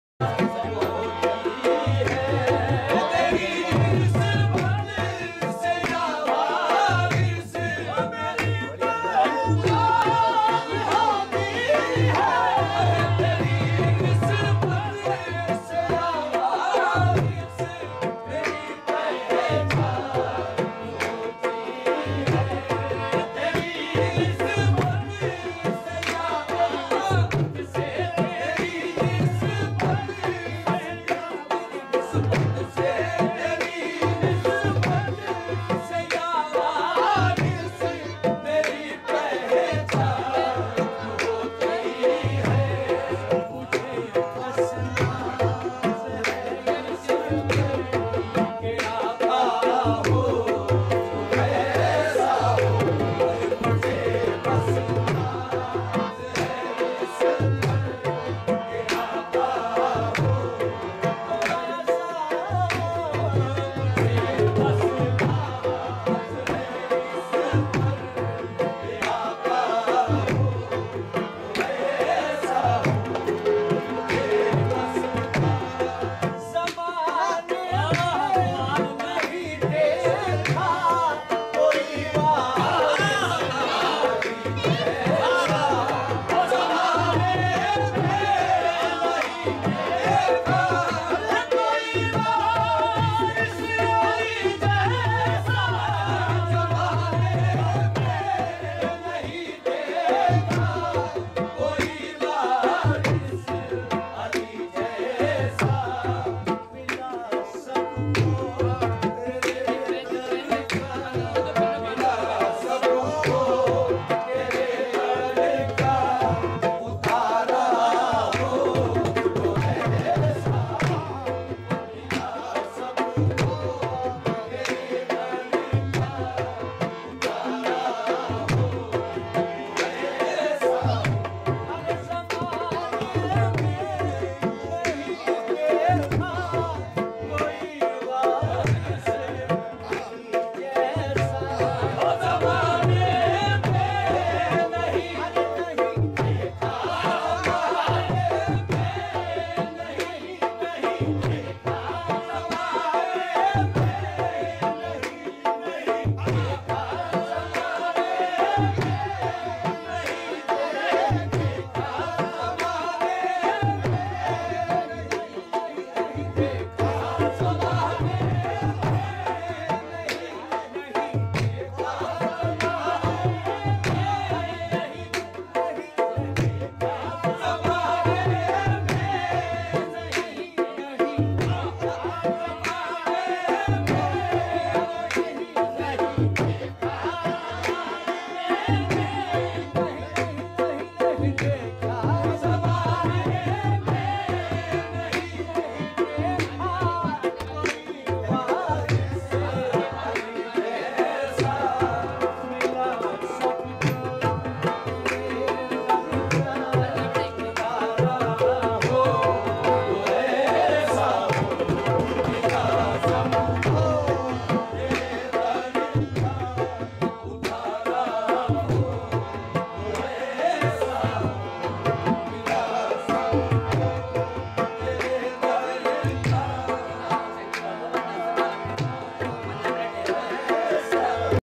jashne waris pak मै nazre waris pak part 14th at home kashish Warsi kath ki puliy kisrol moradabad
जश्न ए वारिस ए पाक
               मैं
नज़रे वारिस पाक part 14th

स्थान : kashish warsi मौहम्मद अली रोड काठ की पुलिया किसरोल मुरादाबाद U.P INDIA

Next vedio  (part-15th) के लिए

                 Or
🔔बैल आइकन दबाएं

कव्वाल : अली वारिस मुंबई